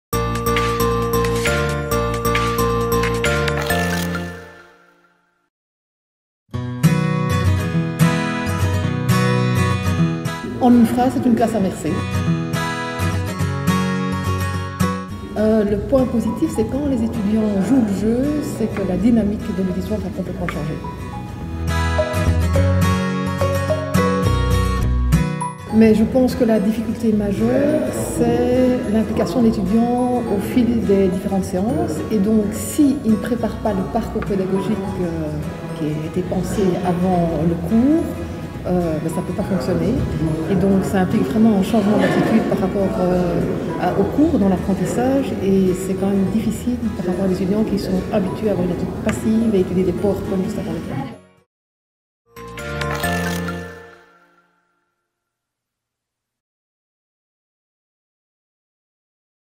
Pratiquer la classe inversée avec ses étudiants | Marthe Nyssens
Le 4 mai 2018, les étudiants et enseignants de l'Economic Scool of Louvain (ESL) et de la Faculté des Sciences Economiques, Politiques Sociale et de Communication (ESPO) ont échangé à propos de pratiques pédagogiques innovantes de 5 d'entre eux. Morceaux choisis!

Marthe Nyssens pratique la classe inversée : les étudiant prennent connaissance du contenu à domicile et présentent au cours les éléments qu'il ont eu pour mission d'approfondir.